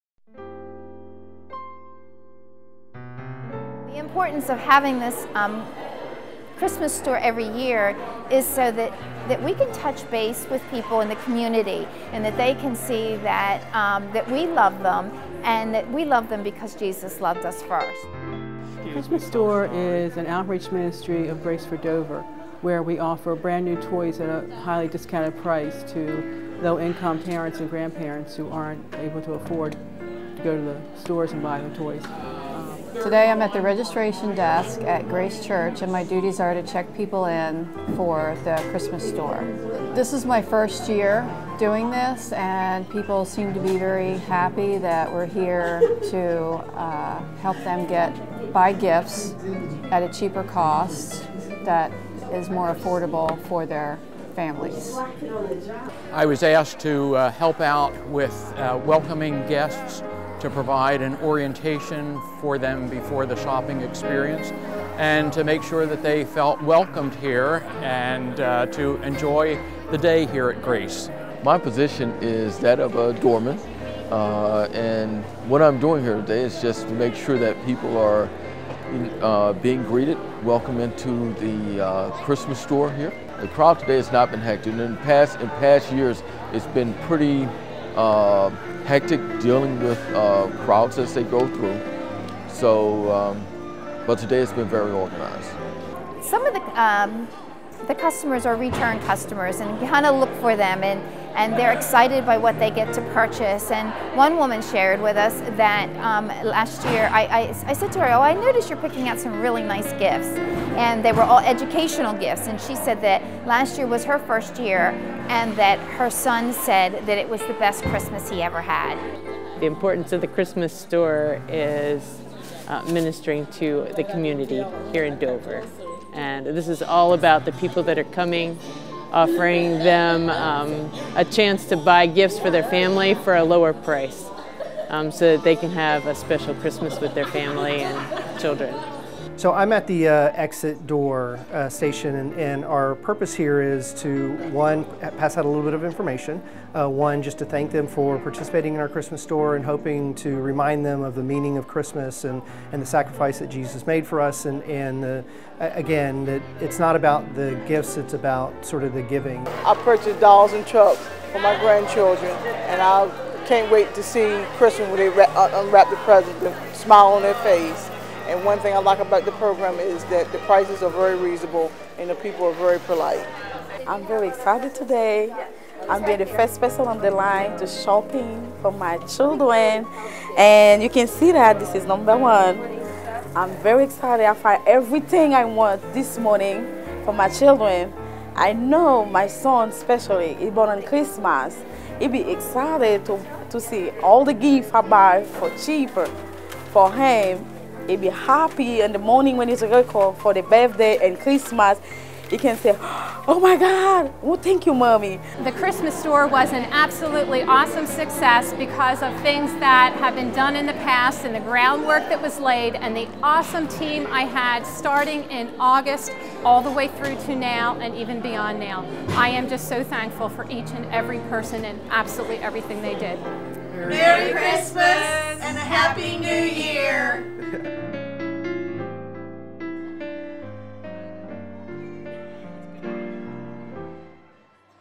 Grace for Dover Christmas Store 2016
What is the Christmas Store and how is it carried out? Here's a video overview of this Grace for Dover initiative held at Grace Church (PCA), Dover, DE.